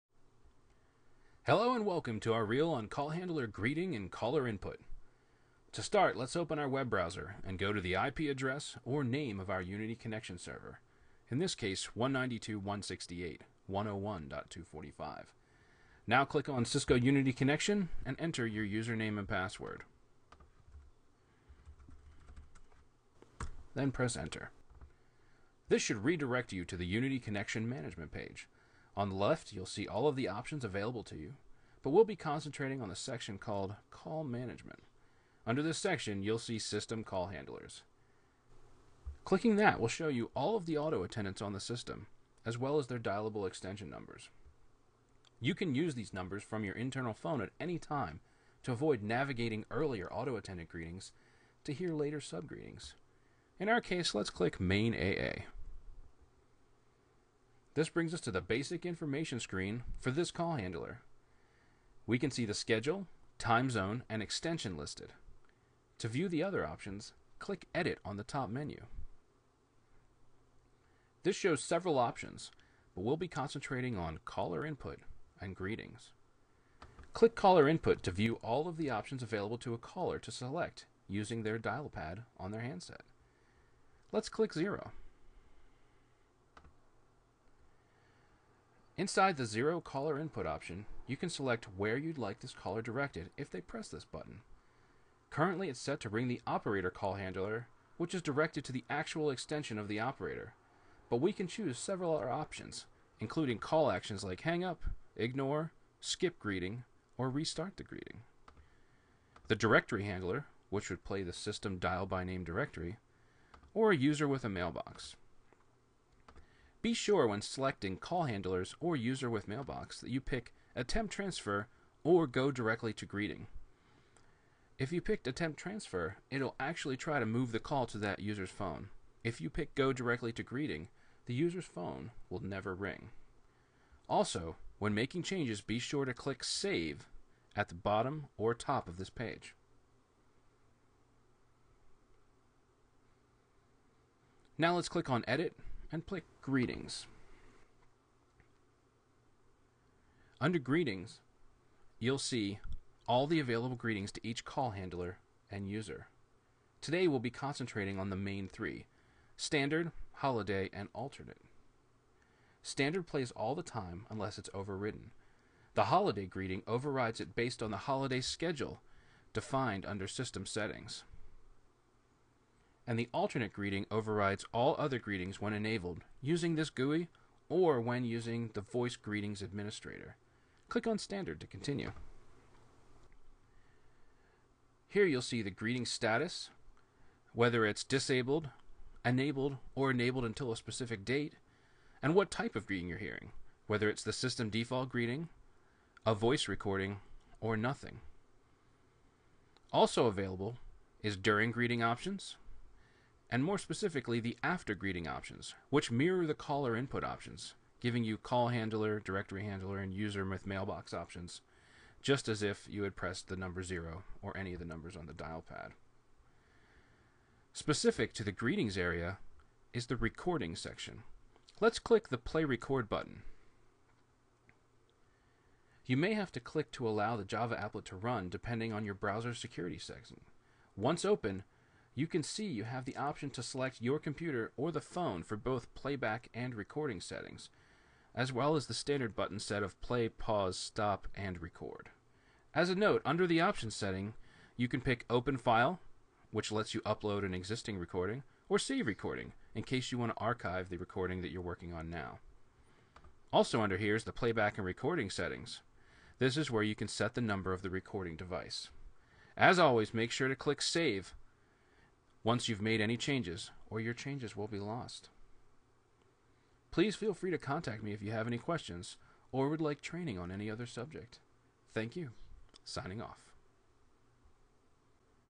CUCN_Greetings and callerinput
Unity Connection 8.5 Call Handler Greeting and Caller Input configuration.